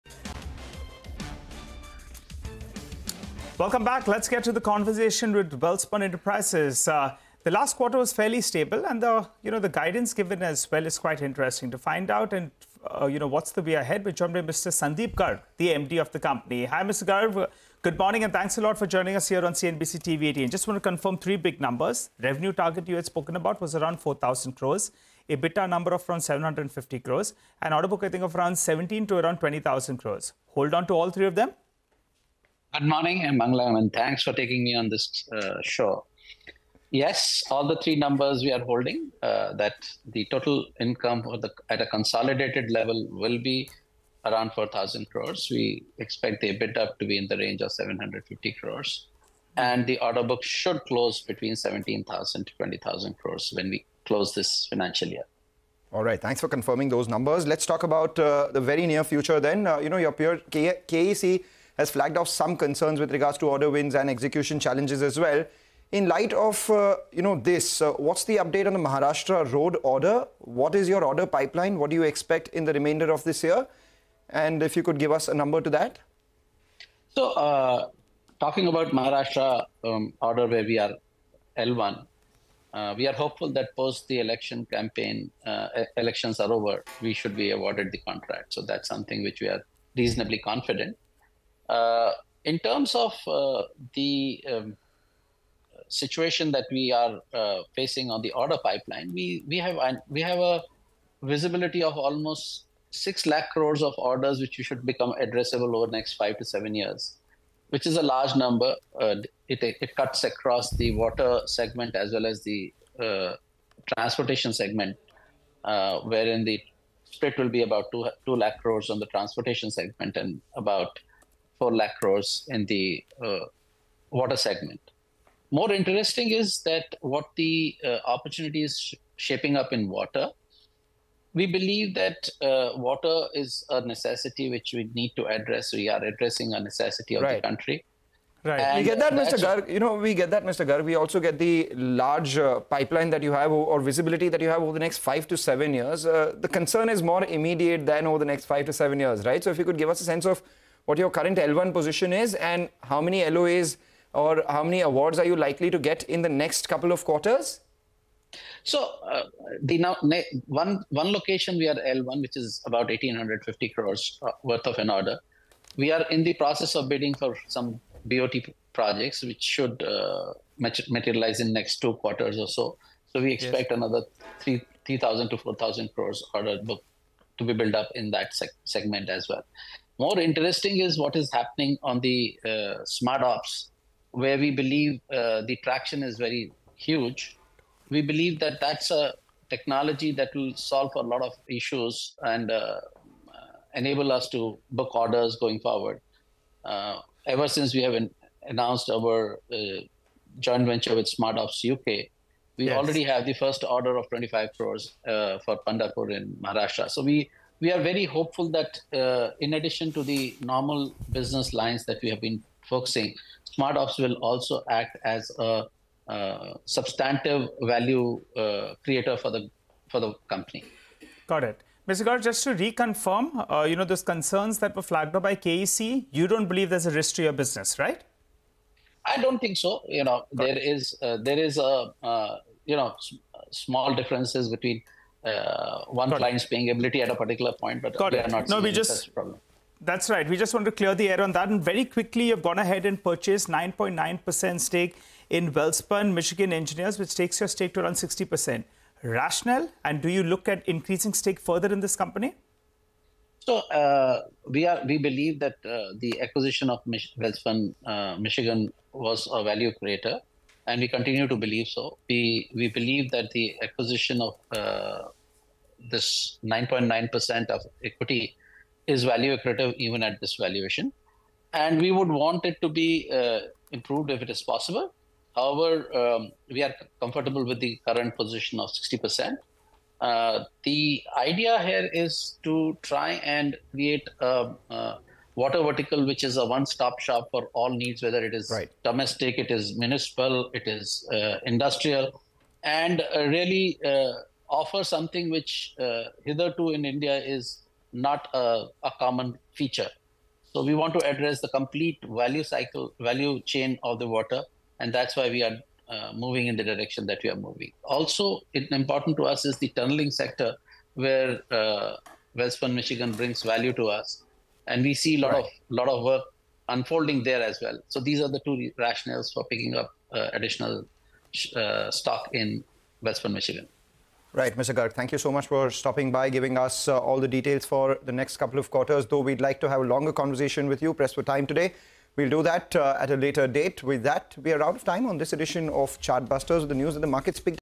Should Be Able To End This Year With Orderbook Of ₹17,000-20,000 Crore: Welspun Ent | CNBC TV18
2QWithCNBCTV18 | Should be able to end this year with orderbook of `17,000-20,000 crore, says Sandeep Garg of Welspun Enterprises. Tells Mangalam Maloo & Nigel D'Souza that the company expects order visibility of Rs 6 lk crore in the next 5 years.

n18oc_business